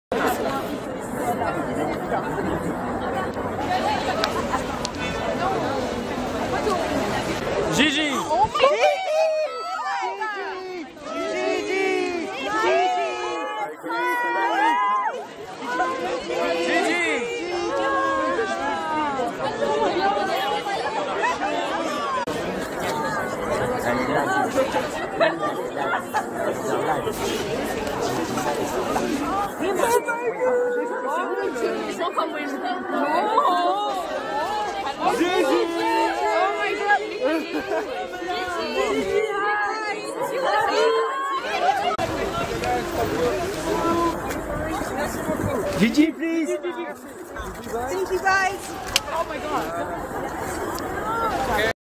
Gigi Hadid - Tommy Hilfiger x Zendaya fashion show - 03.03.2019
The American Top model Gigi Hadid attending the Tommy Hilfiger x Zendaya fashion show in Paris, March 3rd 2019.